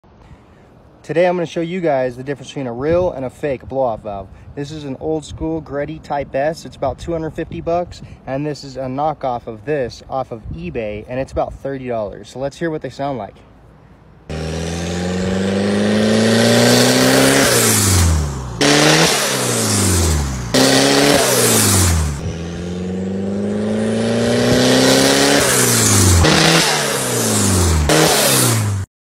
Real VS Fake Greddy BOV Sound Difference 🔊
Real Greddy Type s BOV vs fake ebay knockoff off BOV sound comparison on a Mitsubishi Eclipse GST.